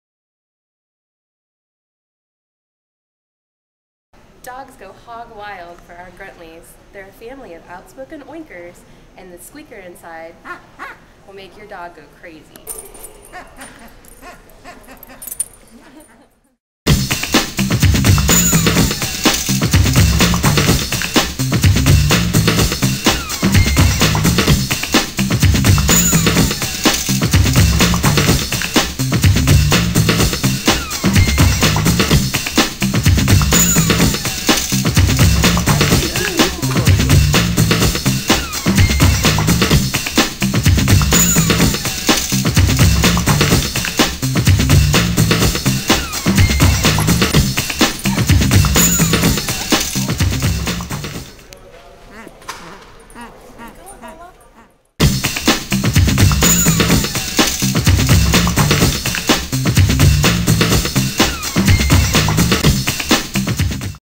Gruntleys
Always ready for a good ol fashioned Grunt-o-Rama, Gruntleys are outspoken oinkers that feature a perfectly-porklike sound device that dogs just cant resist. Forget the bacon  these happy hogs will have your dog beggin for more (and more)! E-I-E-I-O!